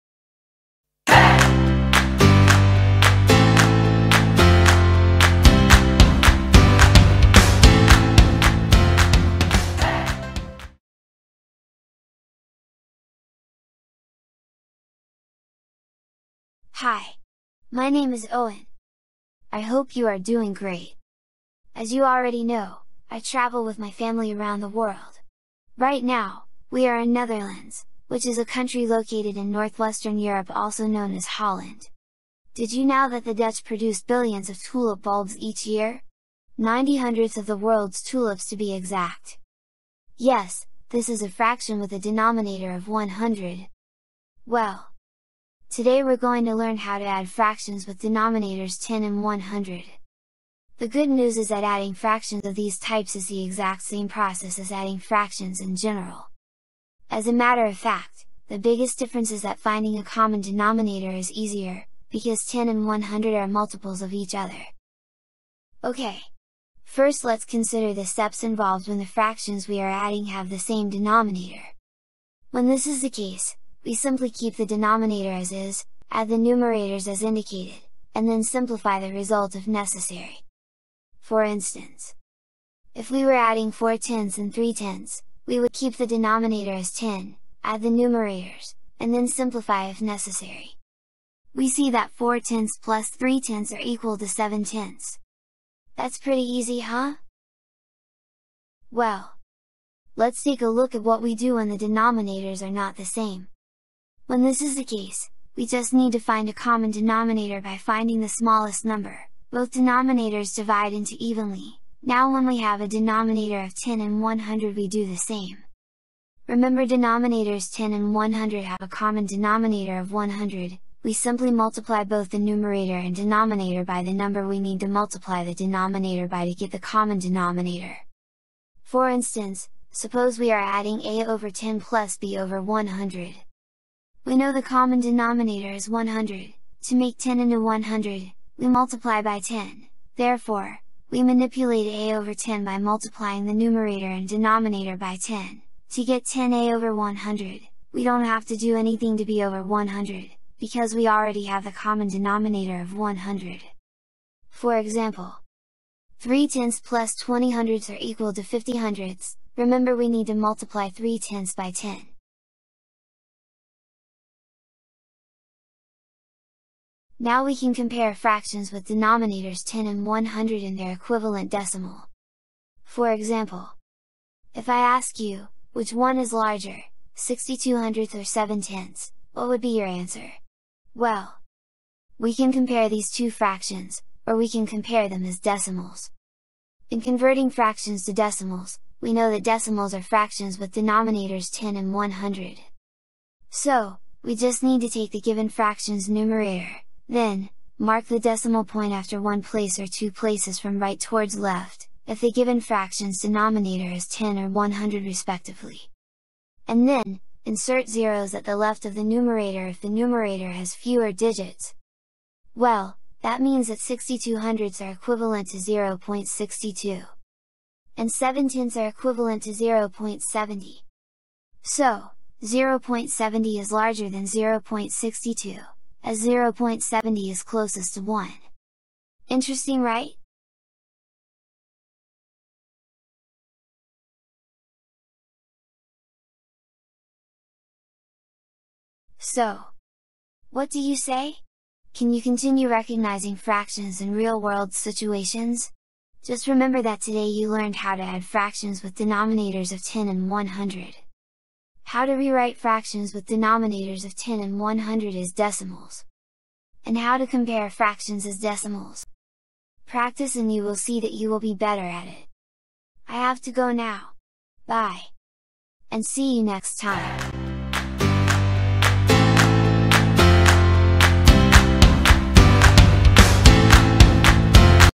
Fractions with denominators of 10 and 100
This is a Mathematics instructional video that walks students through the concept and characteristics of fractions with denominators of 10 and 100.

For a complete lesson plan, a fun interactive game and other educational resources, please visit NUITEQ Snowflake at Snowflake.live .